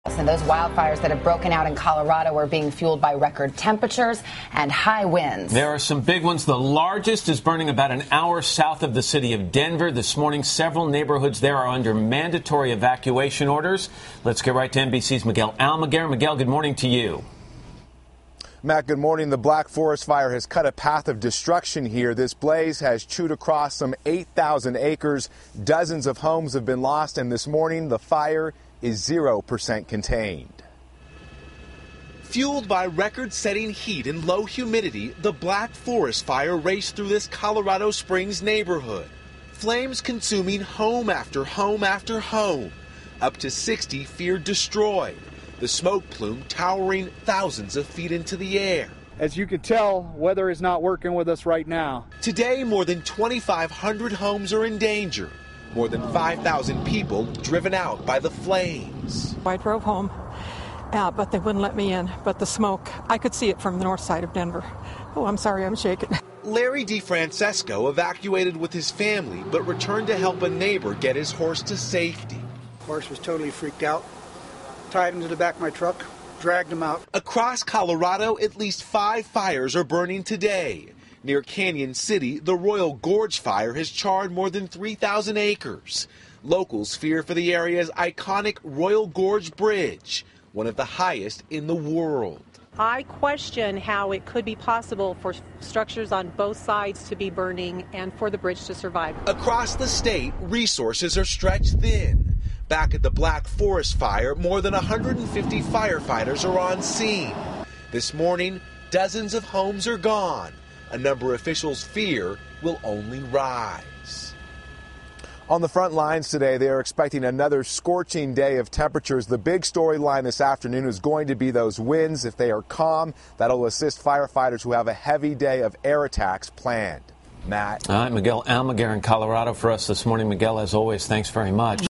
Wildfires threaten homes, char thousands of acres in Colorado
By Matthew DeLuca, Staff Writer, NBC News
A series of wildfires flared up across three separate regions of Colorado on Tuesday, burning across thousands of acres and forcing evacuations as the fires stretched the limits of state and local fire agencies.
A red-flag warning was put into effect for southeastern Colorado through Tuesday night as 95-degree temperatures and strong winds contributed to "critical fire weather conditions," the National Weather Service reported.
The largest of the blazes, the Black Forest Fire in El Paso County, had grown to between 7,500 and 8,000 acres and damaged or destroyed 40 to 60 structures, authorities said.
"As you can tell, weather is not working with us, but our guys are giving it a heck of a shot," El Paso County Sheriff Terry Maketa said. "The challenge we're facing is a lack of manpower."
More than 150 firefighters and 130 law enforcement personnel were at work to quench the flames, which had caused no reported injuries or fatalities.

Official gives an updated on the wildfire near Fremont, Colorado and expresses concern for the Royal Gorge Bridge.
Some residents who had resisted earlier evacuation orders later "changed their minds" as the fire grew, Maketa said. Local fire officials said they were engaged in search and rescue efforts to help those residents on Tuesday night.
"Law enforcement is scrambling to get folks out of there and do searches," Maketa said.
One aircraft and dozens of firefighters worked to contain the Royal Gorge fire near Canon City, Colo., after the blaze was reported around 1 p.m. local time on Tuesday. U.S. Highway 50 and local roads were closed as mandatory evacuations were put into effect for the surrounding area.
The fire had burned through more than 3800 acres and consumed three structures as of Tuesday evening. Containment remained at zero percent as the day ended.
"It's certainly not out of the question that we'll be fighting this fire for another week," said Royal Gorge fire spokesman Gregg Goodland, who added that Canon City itself might still need to be evacuated. "This fire is not going to go away any time soon, especially under the dry conditions."
The Federal Emergency Management Agency approved assistance grant for the Black Forest and Royal Gorge fires, the state's Office of Emergency Management said. Responders would get some support around 8 am local time on Wednesday as the state's emergency operations center would activate to help quench the fires, the emergency management office said.
A fire started by a lightning strike broke out Monday afternoon in a remote section of Rocky Mountain National Park and had grown to between 300 and 400 acres on Tuesday, NBC News affiliate KUSA reported. Park officials usually let naturally occurring fires burn themselves out, but have decided to try to put out what they've dubbed the Big Meadows fire, the station reported.